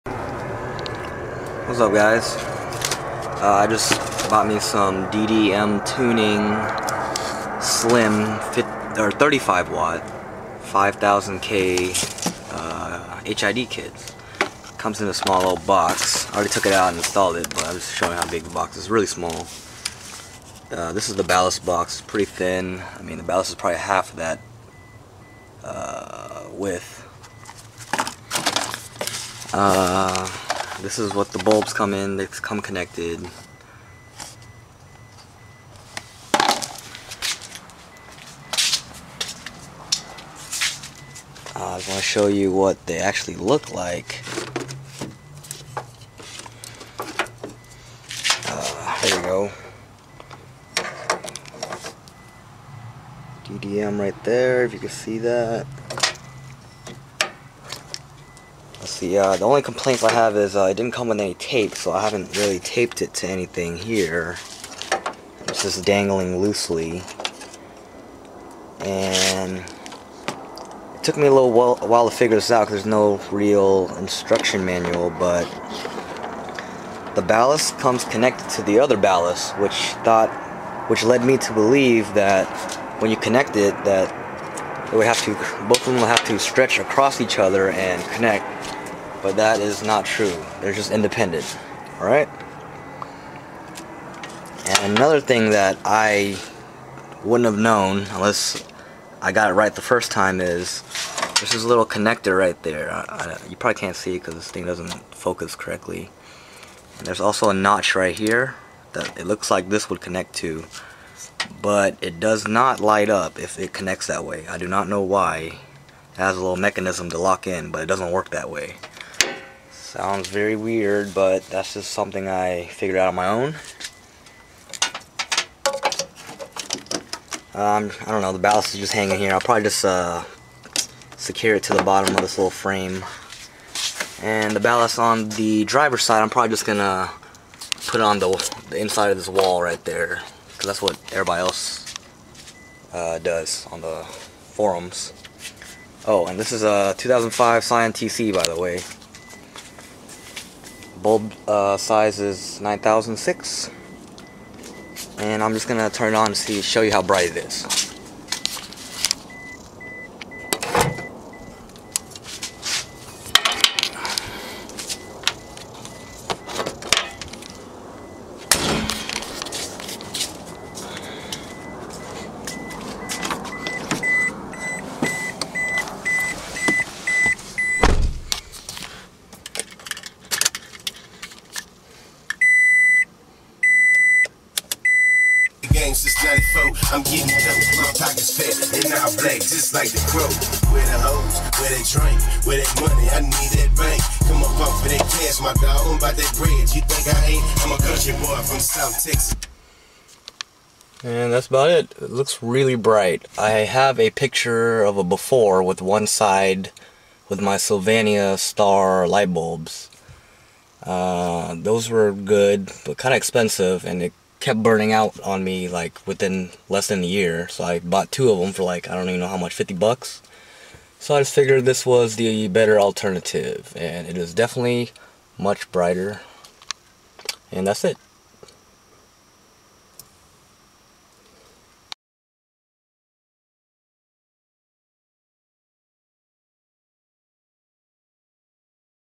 SCION TC DDM Tuning HID slim 5000k tips tricks review and sylvania star comparison
2005 Scion tc 9006.
DDM tuning HID slim kit 5000k 35w no relay. $35 + shipping = $46

much better deal than spending 50 bucks for sylvania star bulbs every 10 months.